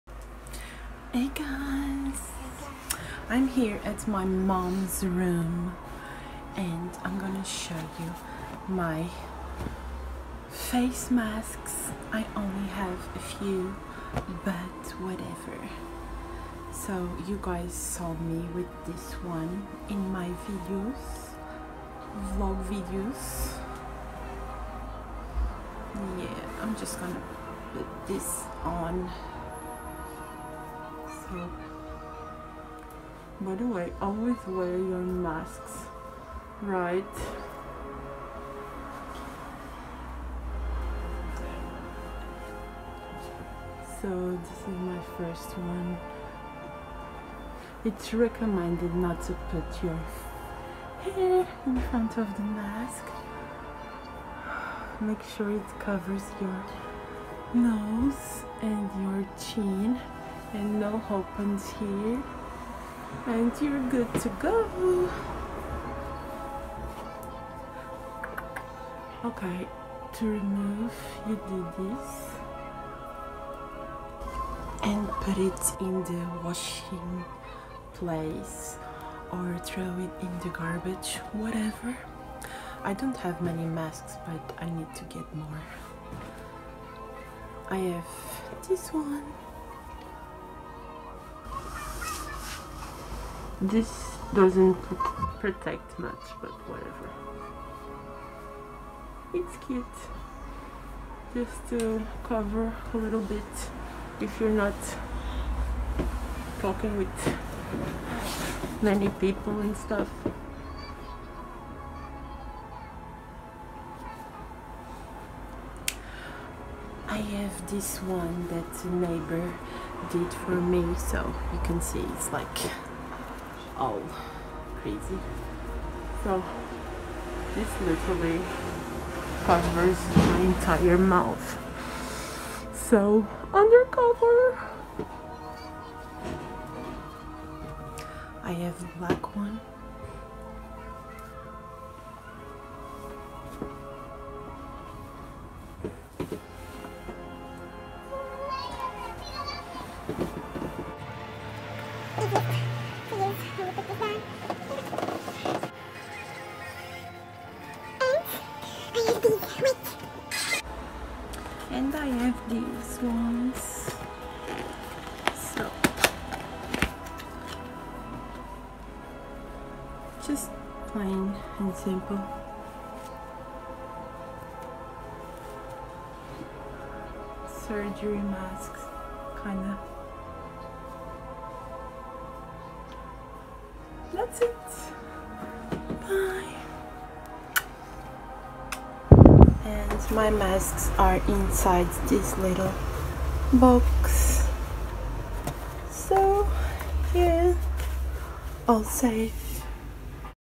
My Covid-19 Facemasks so far - What I’ve been using - Covid / Flu / Safe / Bio / Cybergoth 🦠
My Covid-19 Facemasks so far - What I’ve been using - COVID-19 / Coronavirus / Flu / Safety / Biocare 🦠 ✨